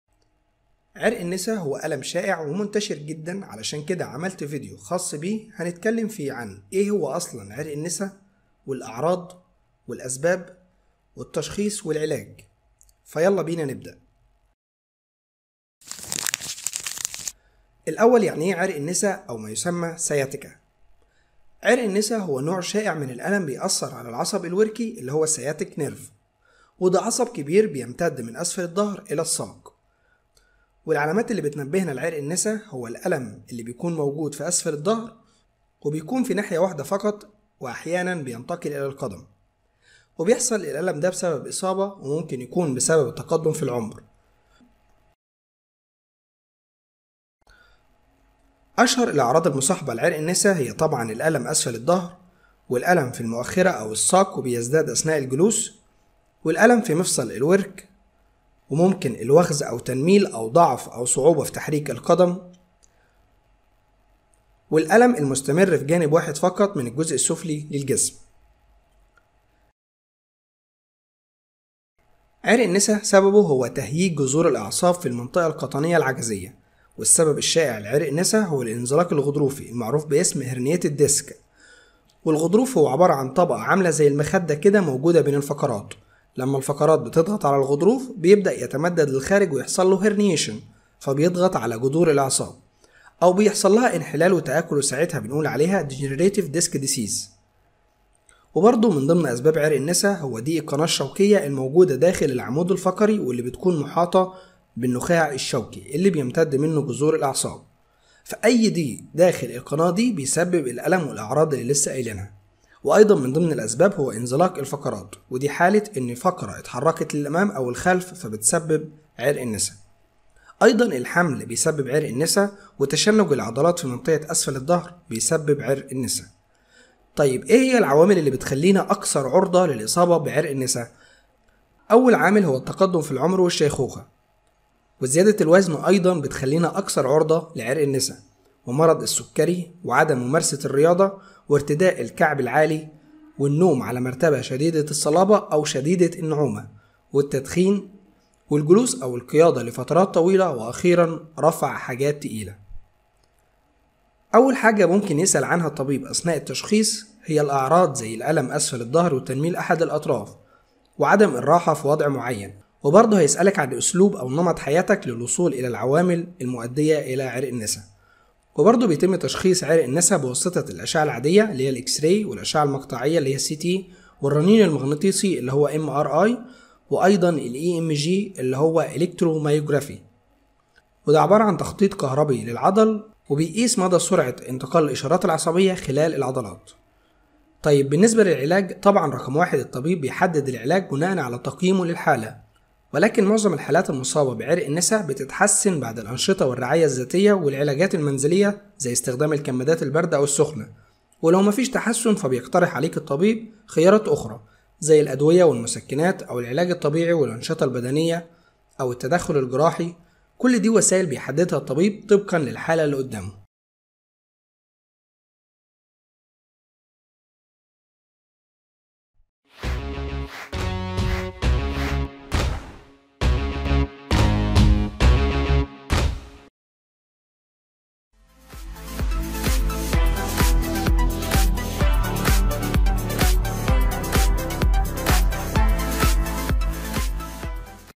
عرق النسا || sciatica
عرق النسا هو الم شائع ومنتشر جدا علشان كدا عملت فيديو خاص بيه هنتكلم فيه عن
ايه هو اصلا عرق النسا
الاعراض
والأسباب
والتشخيص
والعلاج
ف يلا بينا نبدأ

الاول يعني عرق النسا او ما يسمي
Sciatica
عرق النسا هو نوع شائع من الالم بيأثر علي العصب الوركي اللي هو
Sciatic nerve.
ودا عصب كبير ممتد اسفل الضهر اللي الساق .
والعلامات اللي بتنبهنا لعرق النسا هو  الالم اللي بيكون موجود في اسفل الضهر وبيكون في ناحيه واحده فقط واحيانا بينتقل اللي القد م .
وبيحصل الالم دا بسبب اصابه او ممكن يكون بسبب التقدم في العمر.
وممكن في بعض الظروف الموضوع يبقي اسوء.

اشهر الأعراض المصاحبة لعرق النسا هي طبعا الالم اسفل الضهر.
الالم في المؤخره او الساق وبيزداد اثناء الجلوس .
الم في مفصل الورك
وخز او تنميل او ضعف او صعوبه في تحريق القدم .
الم مستمر في جانب واحد من فقط من الجزء السفلي للجسم .

الاسباب والعوامل اللي بتؤدي اللي عرق النسا

عرق النسا سببه هو تهييج جذور الاعصاب في المنطقة القطنيه العجزييه .
والسبب الشائع لعرق النسا هو الانزلاق الغضروفي
Herniated disc.
والغضروف هو عباره عن طبقه عامله زي المخده كدا موجوده بين الفقرات .
لما الفقرات بتتضغط علي الغضروف بيبدا يتمدد للخارج ويحصله
Herniation
فبيضغط علي جدور الاعصاب
او بيحصلها انحلال وتأكل وساعتها بنقول عليها
Degenerative disc disease
وبرضو من ضمن اسباب عرق النسا
هو ضيق القناه الشوكيه الموجوده داخل العمود الفقري والللي بتكون محيطه بالنخاع الشوكي اللي بيمتد منه جذور الاعصاب .
فا اي ضيق داخل القناه دي بيسبب الالم والاعراض اللي لسه قايلنها .
وايضا من ضمن الاسباب هو انزلاق الفقرات .
ودي حاله ان فقره اتحركت للأمام او الخلف فبتسبب عرق النسا .
ايضا الحمل بيسبب عرق النسا .
وتشنج العضلات في منطقه اسفل الضهر.

طيب اي هي العوامل اللي بتجعلنا اكثر عرضه للاصابه بعرق النسا .
اول عامل هو الالتقدم في العمل والشيخويه.
زياده الوزن ايضا تجعلك اكثر عرضه لعرق النسا
مرض السكري
عدم ممارسة الرياضة
ارتداء الكعب العالي
النوم علي مرتبه شديدة الصلابه او شديده النعومه .
التدخين
الجلوس او القياده لفترات طويله .
واخيرا رفع حاجات تقيله .

التشخيص .
اول حاجه ممكن يسأل عنها الطبيب اثناء التشخيص هي الاعراض زي الالم اسفل الضهر
وتنميل احد الاطراف
وعدم الراحه في وضع معين
وبرضو هيسألك عن اسلوب نمط حياتك للوصول الي العوامل .
وبرضو بيتم تشخيص عرق النسا بواسطه الاشعه العاديه  والاشعه المقطعيه والرنين المغناطيسي.
وايضا ال
EMG
اللي هو
Electromyography
تخطيط كهربيه العضل واللي بيقيس مدي سرعه انتقال الإشارات العصبيه خلال العضلات .

طيب بالنسبه للعلاج .
طبعا رقم واحد الطبيب بيحدد العلاج بناءا على تقيمه للحاله .
ولكن معظم الحالات المصابه بعرق النسا بتتحسن
بعد الانشطه والرعايا الذاتيه والعلاجات المنزليه زي استخدام الكمدات البارده او السخنه .
ولو مفيش تحسن فبيقترح عليك الطبيب خيارات اخري .
زي الادويه والمسكنات .
او العلاج الطبيعي والانشطه البدنيه
او التدخل الجراحي .
كل دي وسائل بيحددها الطبيب طبقا للحاله اللي قدامه .